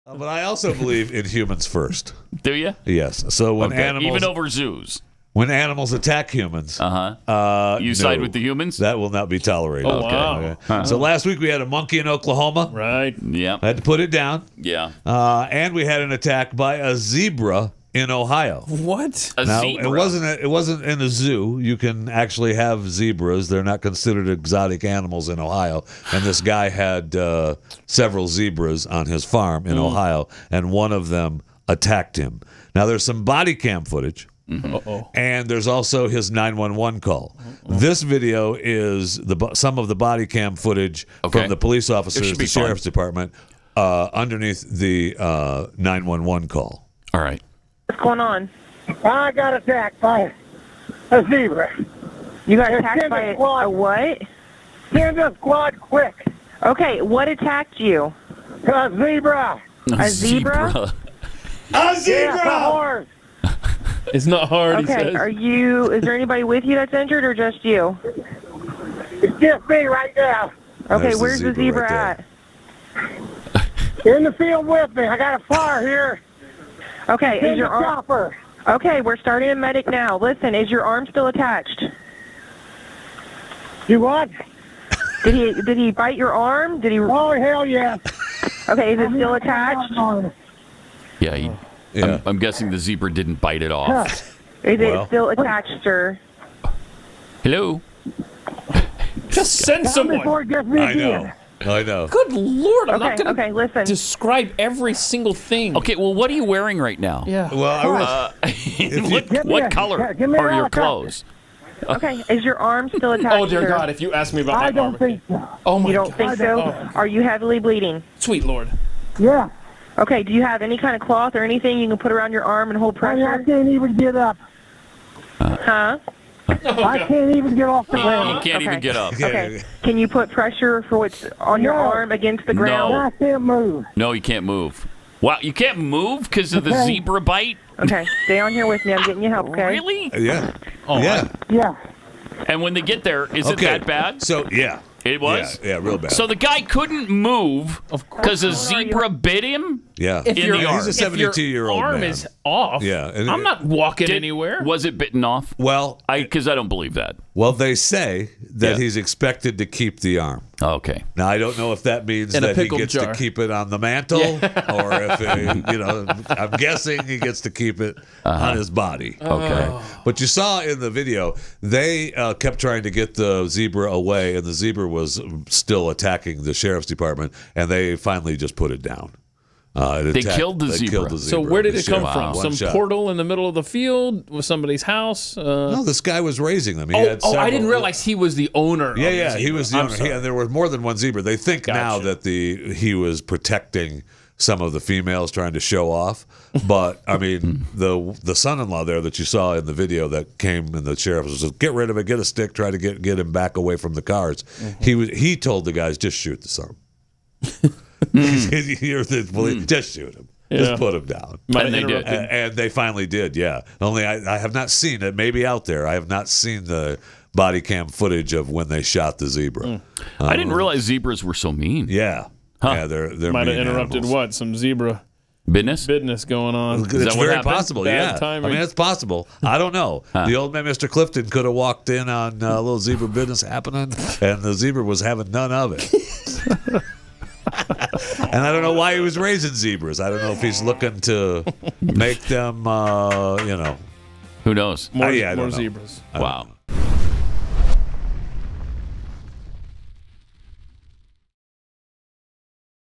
Wild Zebra Almost Bites Man’s Arm Off | Pat Gray
A man had a wild encounter with a zebra that almost bit his entire arm off. After a very awkward call with 911 dispatch, the police arrived and put down the crazed zebra.